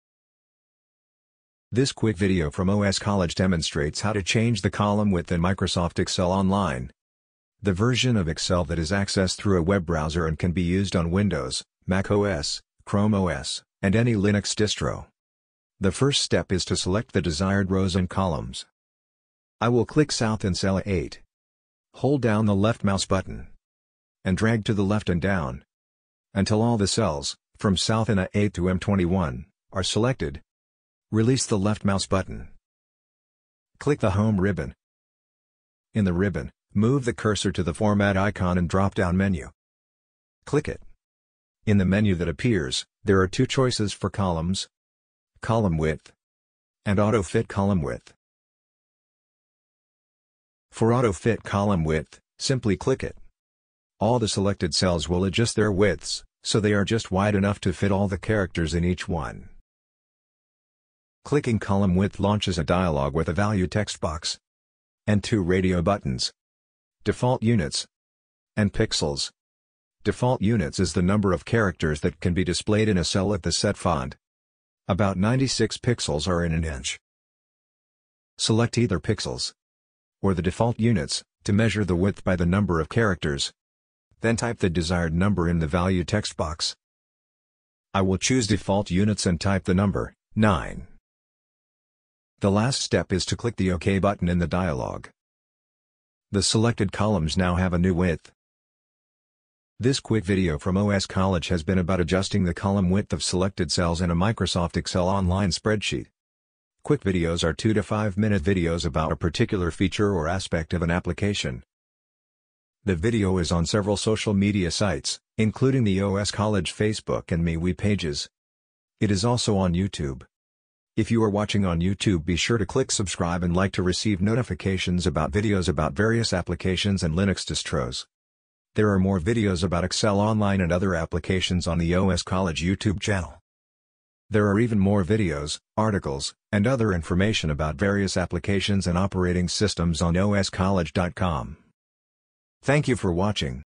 Excel Online Quick Video: Adjusting column width
This video demonstrates two ways to adjust the column width of a selection of cells in Microsoft Excel Online, the version of Excel that is accessed through a Web browser.